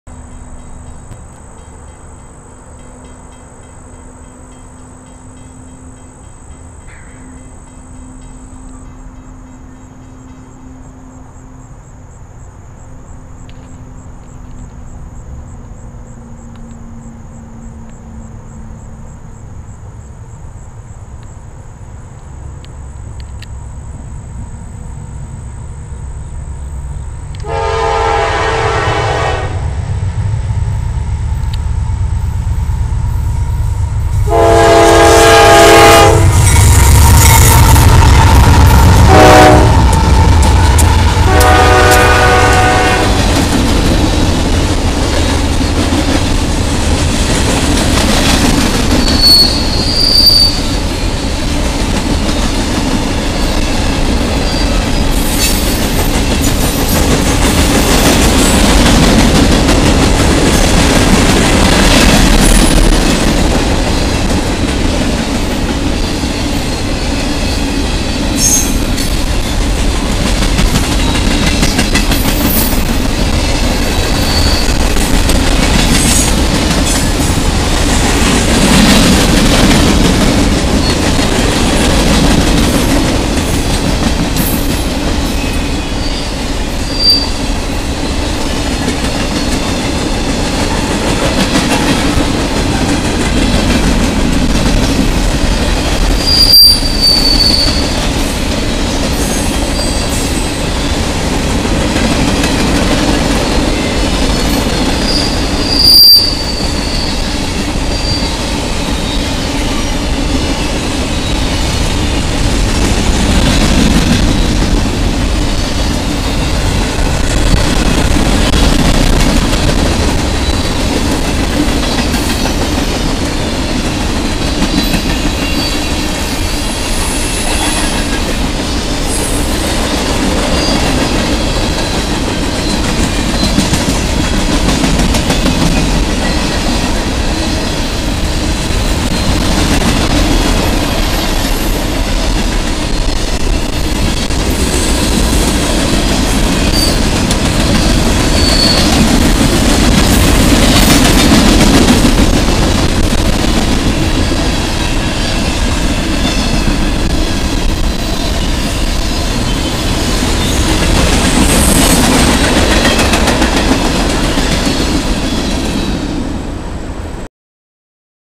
Another Monster Of A Train With A Nice Doppler Effect. 9 - 20 - 2010
Another monster of a grain train makes the turn coming into Marmet on a hot Monday afternoon. As I was returning from a venture over on NS, I noticed a westbound had a "clear" signal at the control point about five miles from town. After seeing the signal, I decided to head to a spot where I knew I could get him without any obstruction blocking my view of the train coming by.  However, I actually thought that he had slowed down between the control point signals at Chelyan and the ABS signal at Chesapeake. Boy, was I dead wrong on that
aspect of it.

I did not have to wait long for this man to show up either. I had not been there for twenty minutes when I started hearing him calling out the signals. By the time that he called out Chesapeake, the ABS signal about two miles to the east, I had already had my camera on the tripod and ready to go. When he came around the curve, I got some really nice doppler effect from where he hit the horn. As with most train that come around that curve, you always get some nice doppler effect because of the retaining wall and the overpass.

Train G871-18 is seen with four locomotives and one hundred and twenty two grain cars.